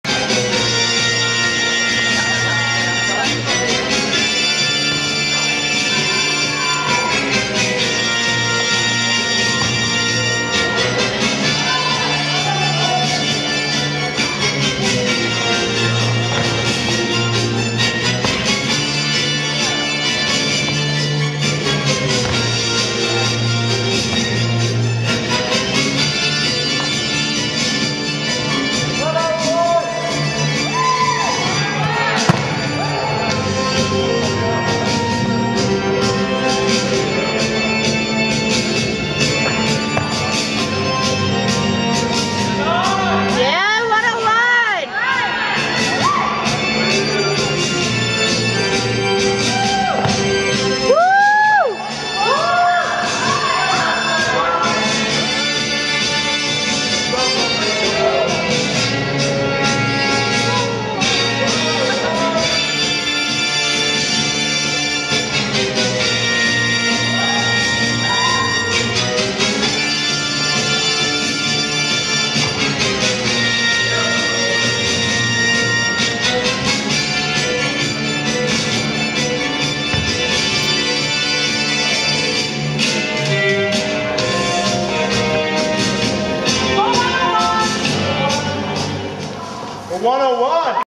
Novice Tango Final
Michael and Amelia Novice Tango Final at Northwestern